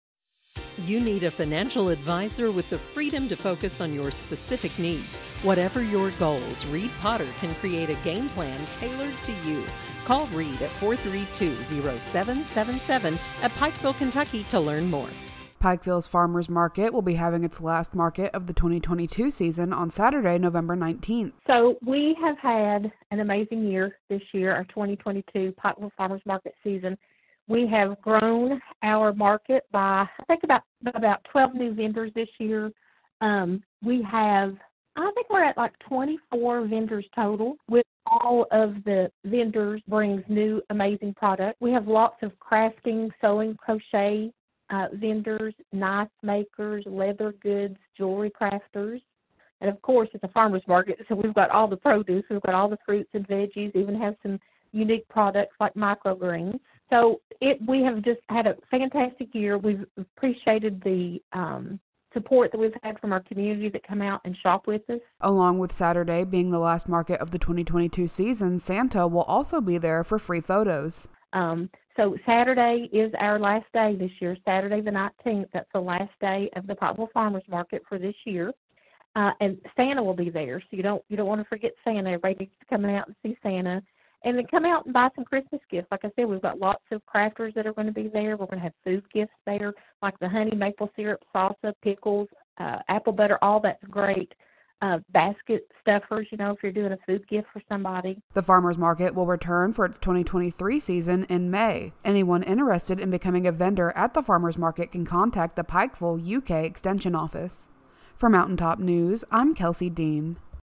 Last 2022 Pikeville Farmer's Market Set for November 19
The final Pikeville Farmer's Market for the year is scheduled for Saturday, November 19.